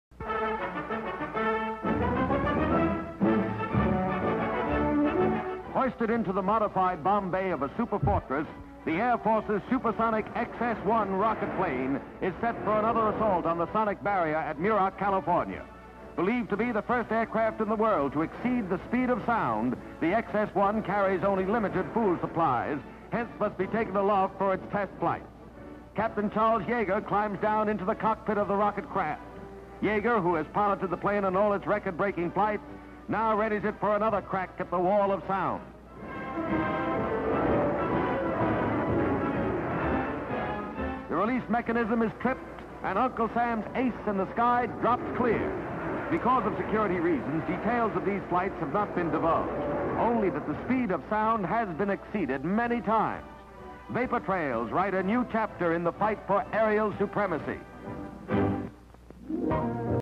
Bell XS-1 Breaks Sound Barrier - 1947 | Today In History | 14 Oct 17
On October 14, 1947, Air Force test pilot Charles E. (“Chuck”) Yeager broke the sound barrier as he flew the experimental Bell XS-1 (later X-1) rocket plane over Muroc Dry Lake in California.